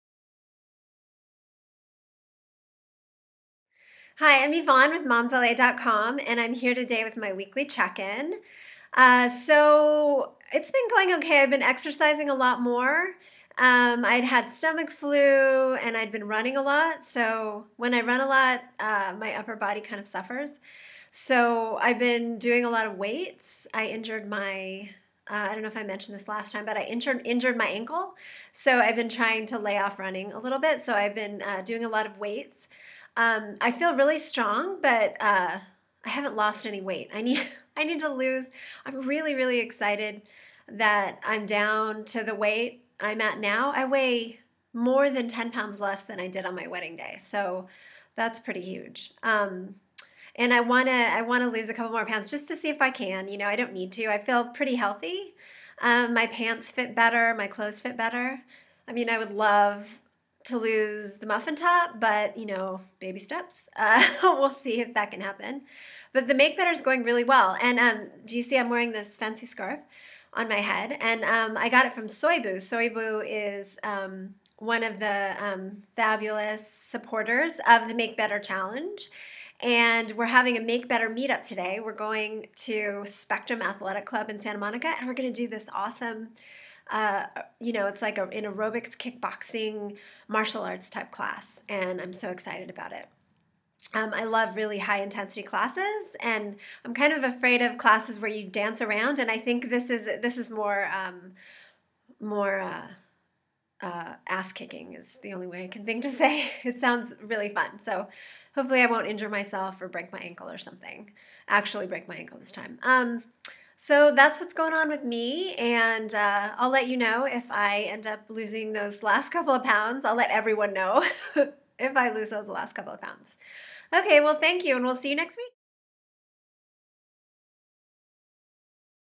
Yvonne's MakeBetter Update for February 15, 2013
Yvonne talks about she's doing this week for the MakeBetter Challenge.

Want to join MomPulse?